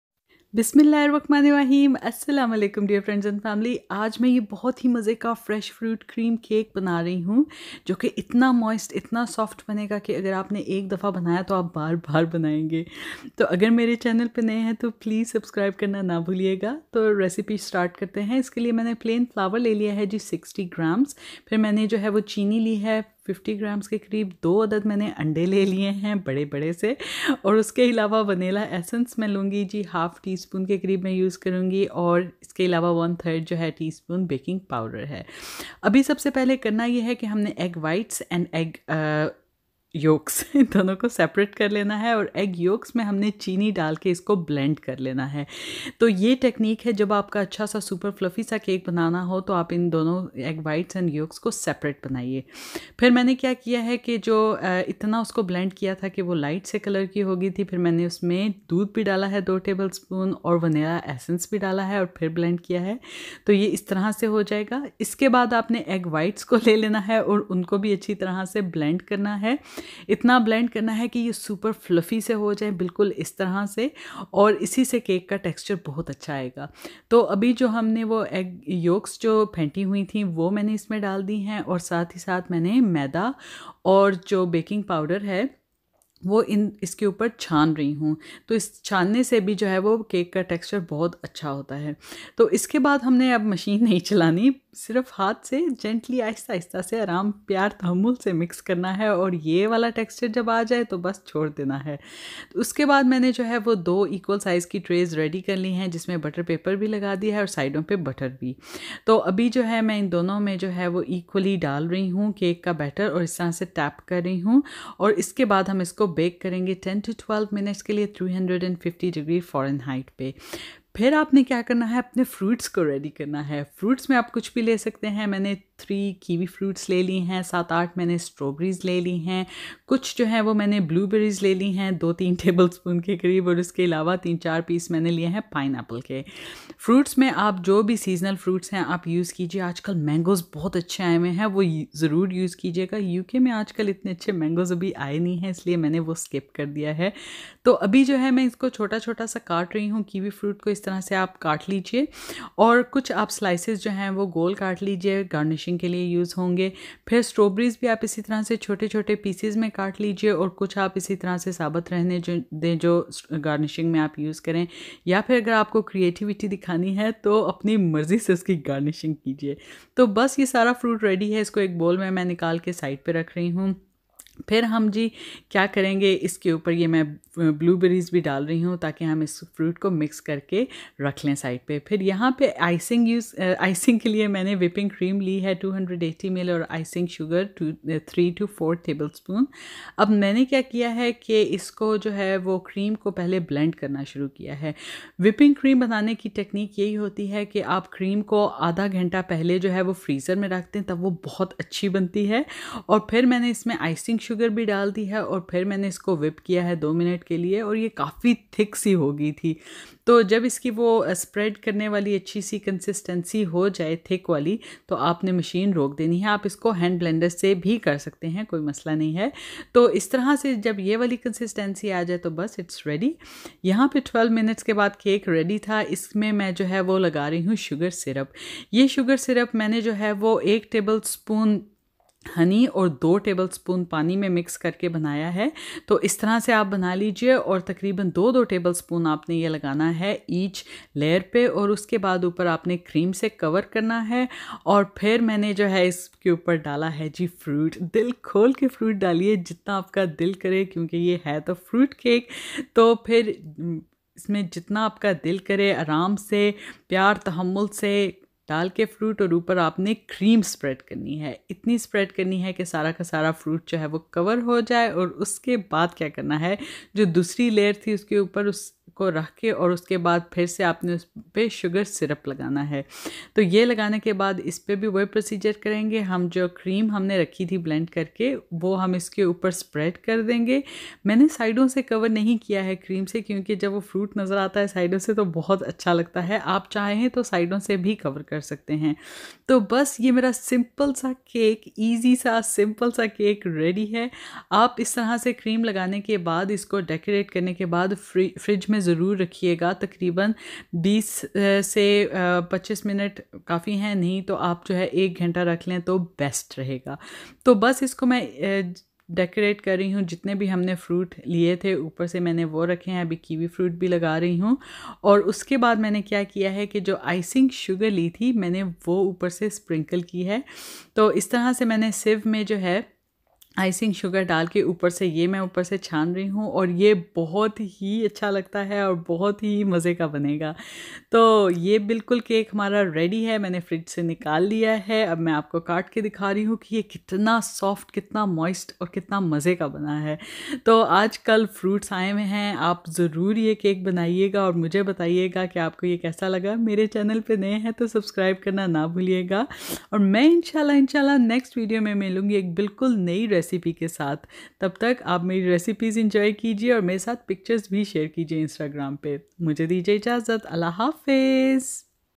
FRUIT FRESH CREAM CAKE/ HOW TO MAKE CREAM CAKE AT HOME IN 15 MINUTES
salam everyone! Todays cake is made with lots of fruits and some simple ingredients. its a fresh fruit cream cake which is soft moist and yummy. you can make it with fruits of your choice and enjoy it on tea parties, birthday parties, eid or get together. Enjoy tye recipe and stay blessed.
Ingredients
CAKE BATTER
Plain flour 60g
sugar 50g
Baking powder 1/3 tsp
Eggs 2
FRUIT
kiwi fruit 3
strawberries 8
Blueberries 2 tbsp
pineapple slices 3
ICING
Whipping cream 280ml
icing sugar 4 tbsp